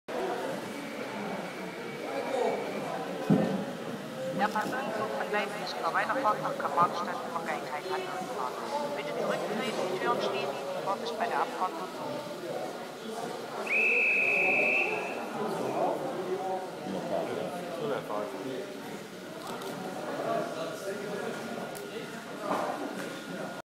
V200 der DDR
Als Leihgabe von Peter G. aus Jena war in Lüdenscheid bei der Ausstellung im Oktober 2014 diese V200 (Reichsbahn der DDR) zu sehen.
Die ganz spezielle Bahnhofsansage hat uns begeistert, hört selber ...